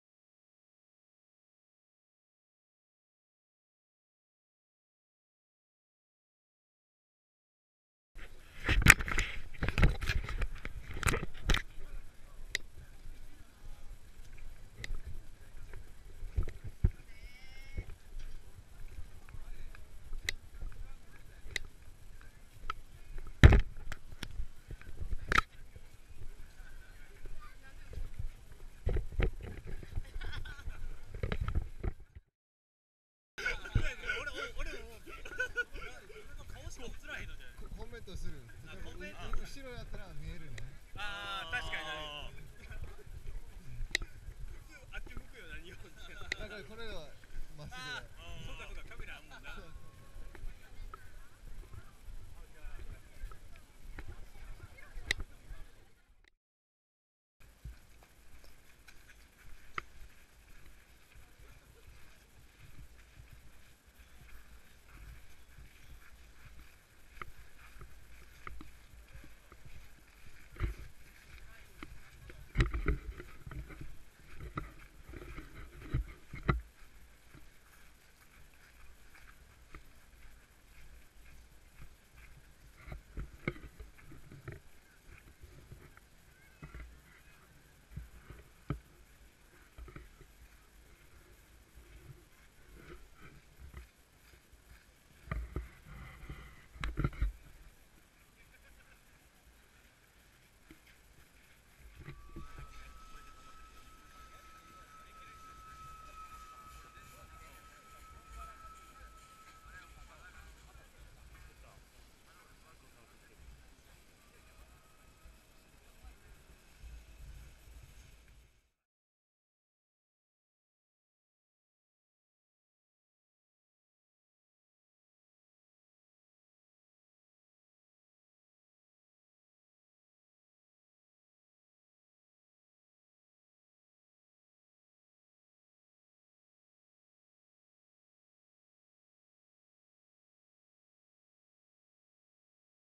平安神宮 Heian Jingu Shrine 2014
New Year's at Heian Jingu Shrine, Kyoto